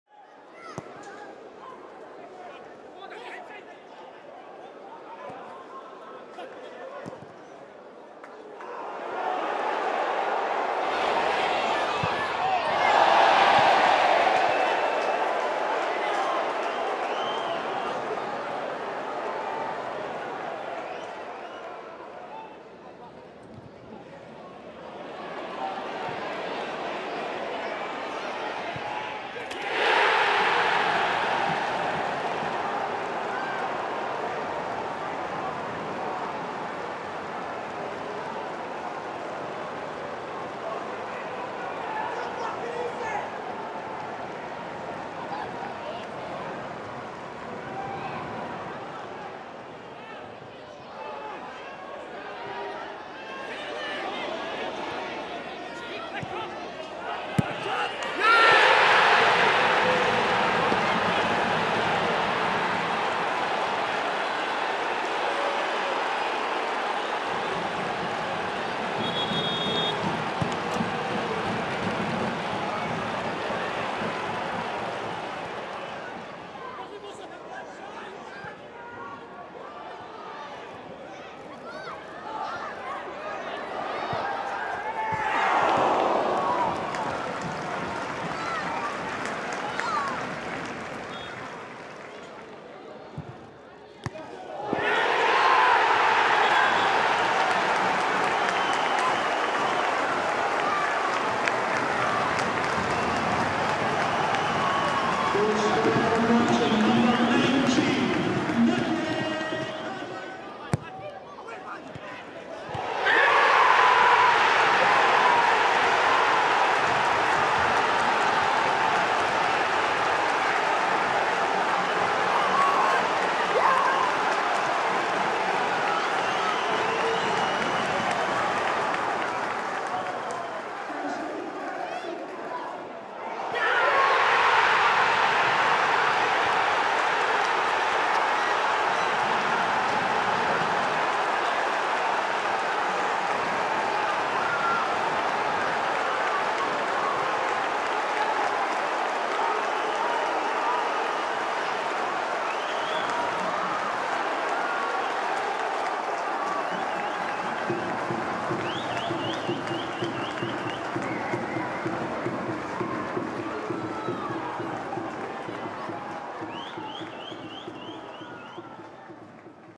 Greenock Morton 3-2 Partick Thistle | Morton stage stunning comeback! | Ladbrokes Championship
Brian McLean's late header capped a wonderful Morton comeback victory against Partick Thistle.